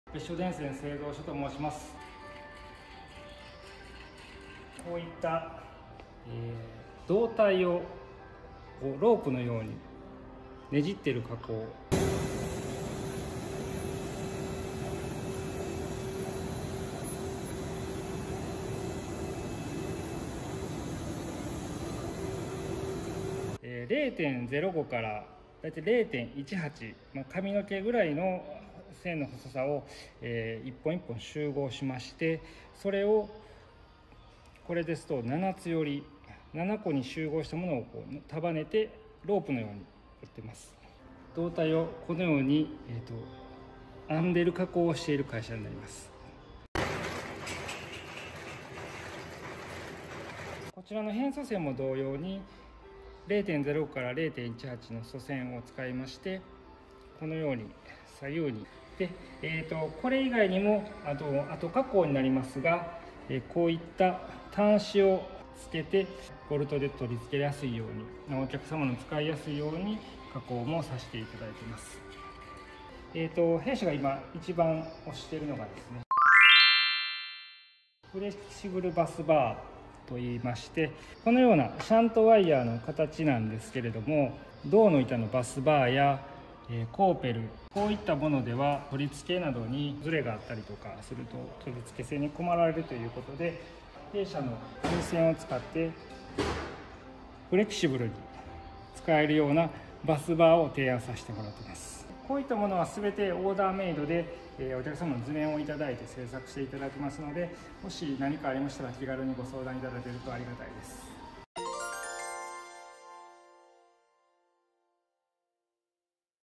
取付位置ズレ解消、振動対策に便利な導体 / Conductor good for eliminating mounting misalignment and preventing vibration
●極細線を撚って編んで作る柔軟性ある電気用導体
株式会社別処電線製造所
●Flexible electrical conductor made by twisting and braiding ultra-fine wires
BESSHO ELECTRIC WIRE MANUFACTORY CO., LTD.